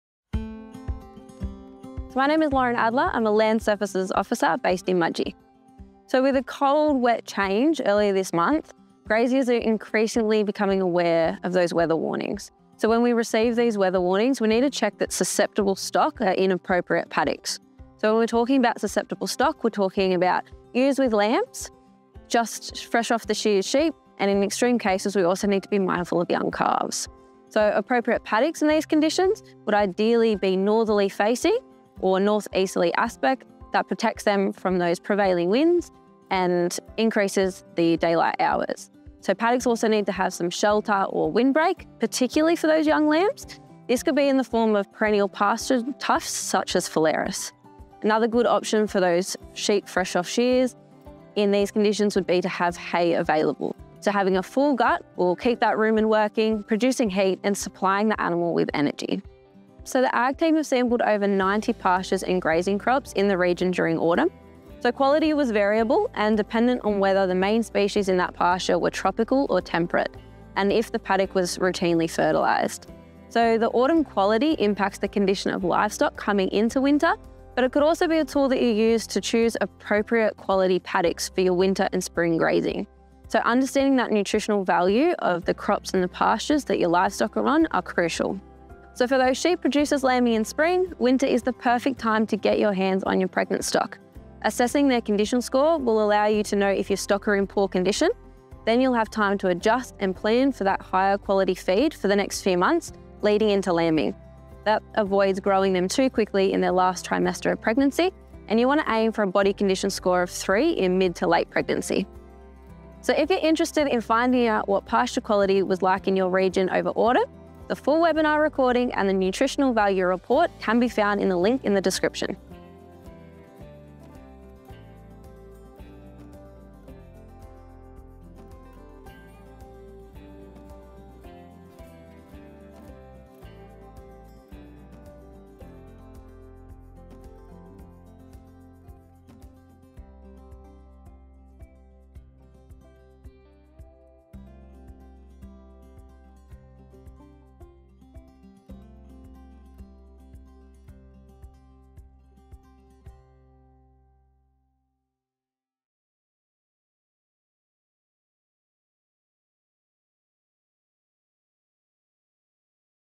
Winter 2024 seasonal conditions | Livestock management advice
Old man winter is here, so it’s time to think about how to best care for your livestock over the colder months. Our Agriculture Land Services Officer Lauren Adler highlights some of the stock management actions you should be thinking about now…

- the best paddocks for susceptible stock (ie ewes with lambs, freshly shorn sheep or even young calves)
- feeding out hay to protect your livestock
- paddock selection and the importance of understanding pasture nutritional value
- what pregnant stock in particular need right now.

You can read the full webinar recording of the nutritional value report